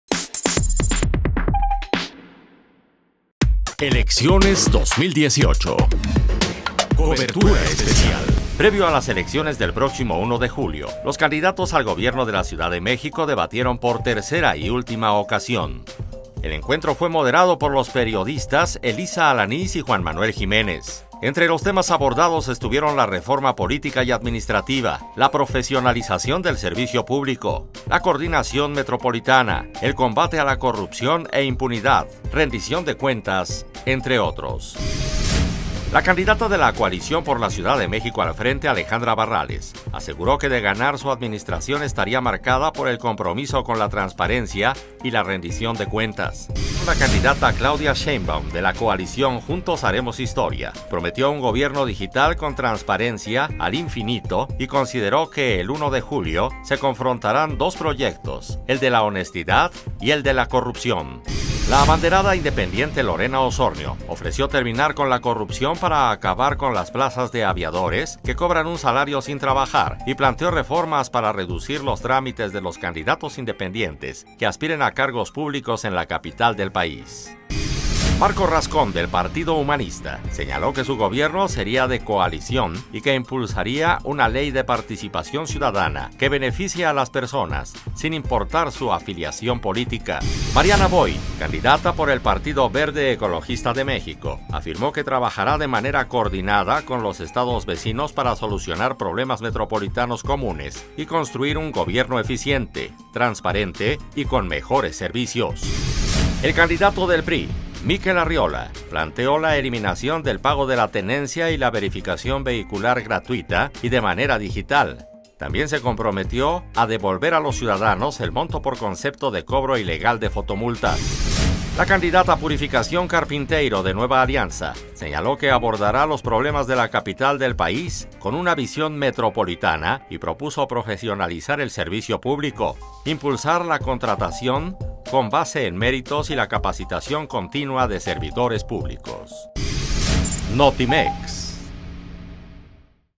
Resumen de actividades de candidatos al gobierno de la CDMX 21 de junio
Previo a las elecciones de próximo 1 de julio, los candidatos al gobierno de la Ciudad de México debatieron por tercera y última ocasión.